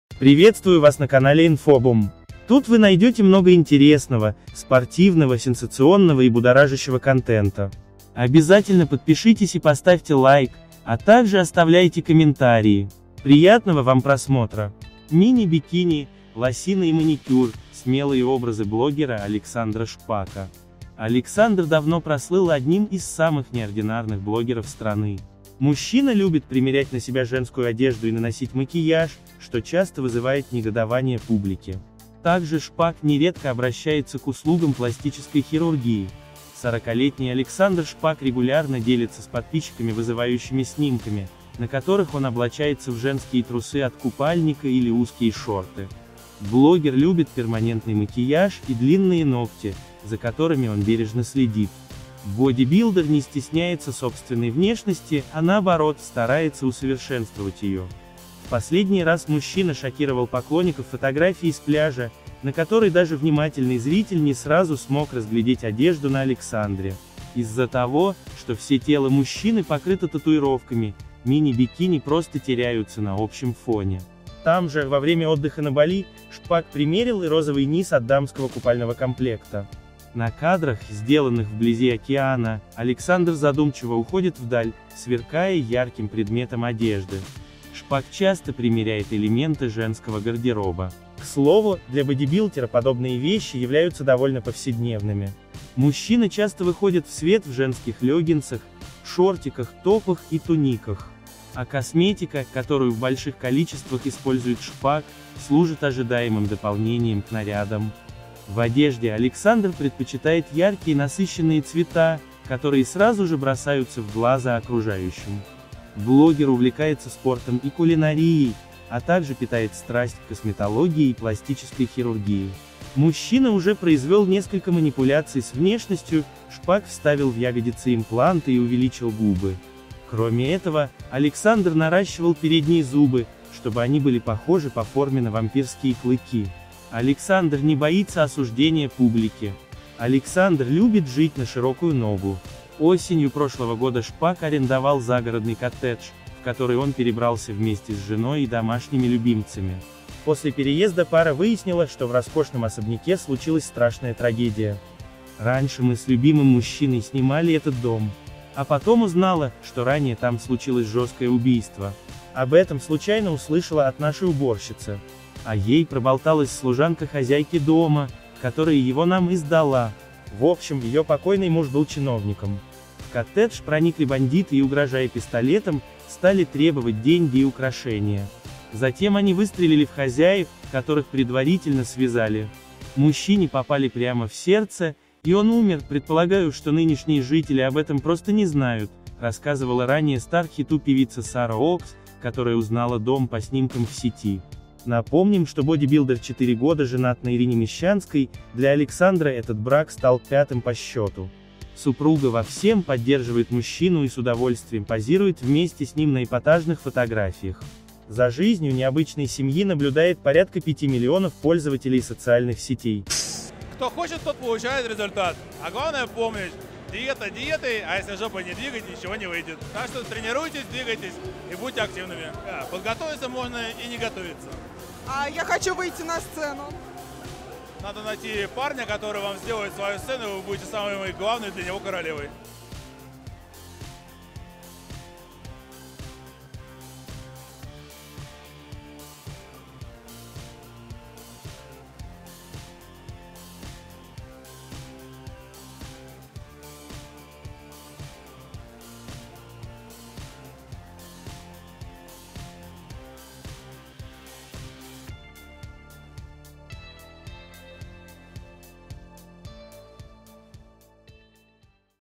Мини-бикини, лосины и маникюр: смелые образы блогера Александра Шпака
На данном видео идет речь об своеобразном блогере которого зовут Александр Шпак, он давно прослыл одним из самых неординарных блогеров страны. Мужчина любит примерять на себя женскую одежду и наносить макияж, что часто вызывает негодование публики. Также Шпак нередко обращается к услугам пластической хирургии.